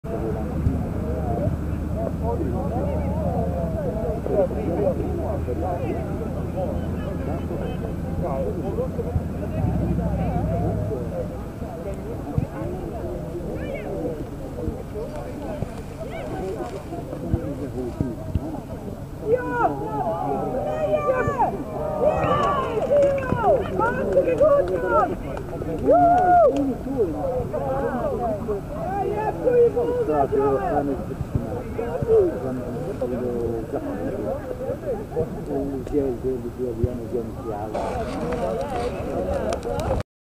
3de Goal van OVCS C1
DVO C2 - OVCS C1, DERDE Goal van OVCS C1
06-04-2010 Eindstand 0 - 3
© Eight II Miracle & 2010 Google - You Tube